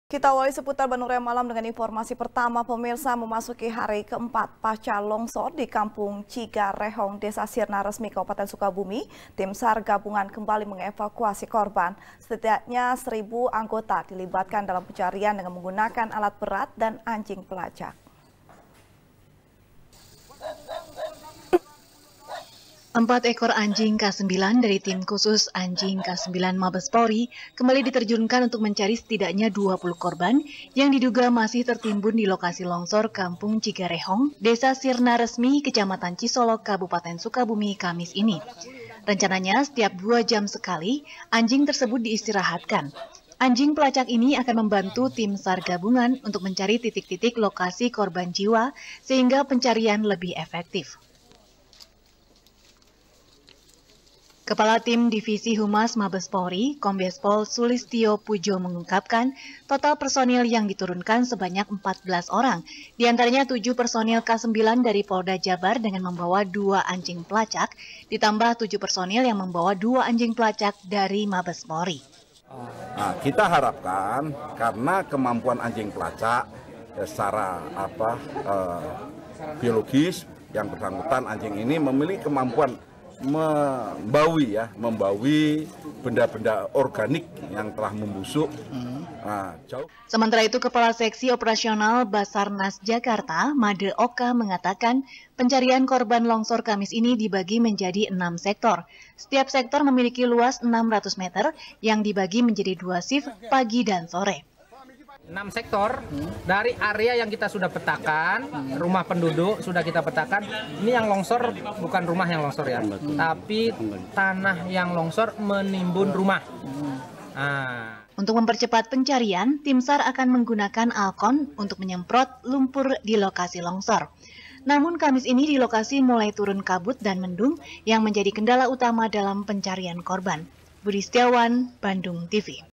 Anjing Pelacak Diturunkan Untukbantu Evakuasi Korban Longsor | SBR | BANDUNGTV
SBR | BANDUNGTV | FARHAT MUMTAZ | Memasuki hari keempat pasca longsor di kampung cigarehong, desa sirna resmi, kabupaten sukabumi, tim sar gabungan kembali mengevakuasi korban. setidaknya seribu anggota dilibatkan dalam pencarian, dengan menggunakan alat berat dan anjing pelacak.